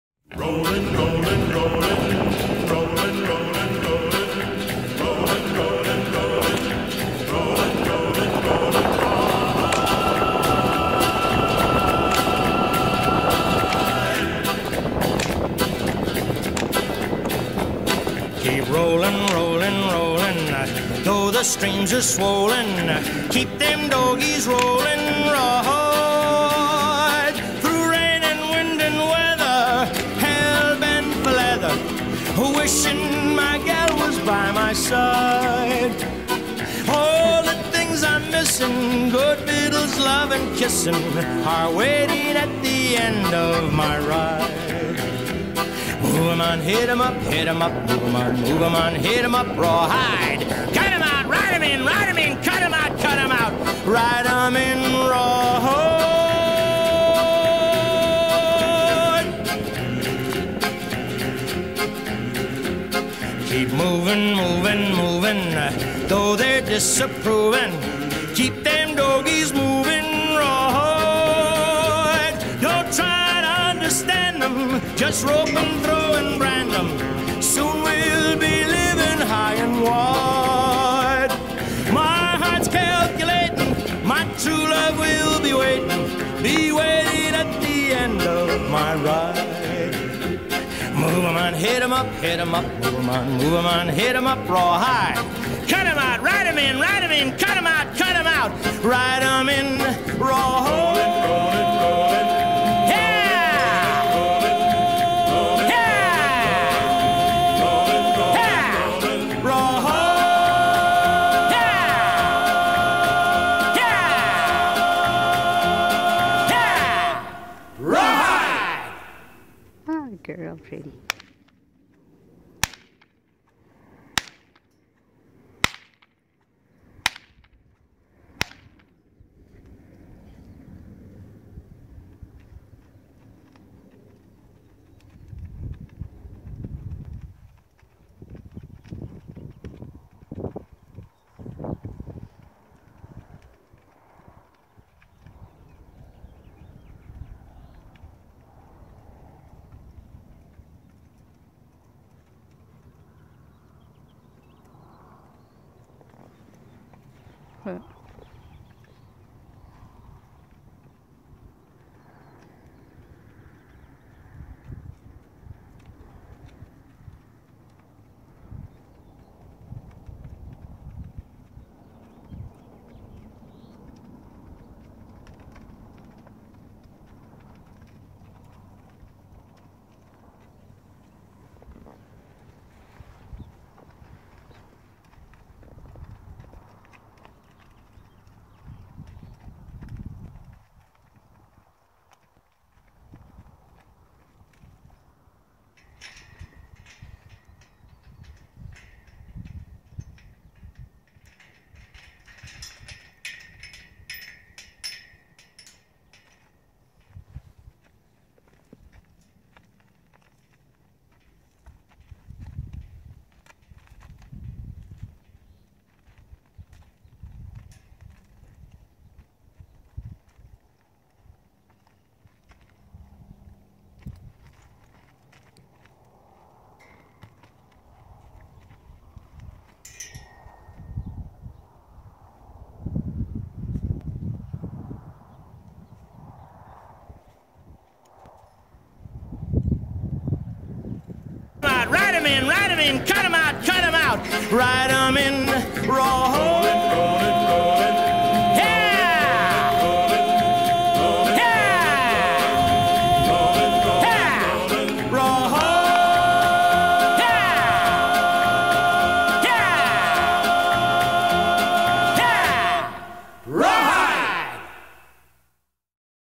2000 Handsome APHA Sorrel Overo Paint Gelding For Sale
2000 Handsome, solid trained sorrel overo blue eyed gelding. Calico is nicely marked and a solid build. A Great minded all around gelding, who guides beautifully and is smooth and responsive to ride. Big stop, nice turnaround, circles great. This handsome gelding has been used in our lesson program. he is a lot of fun to ride and really enjoys his job. He is very personable and great to work around. More pictures and video to follow.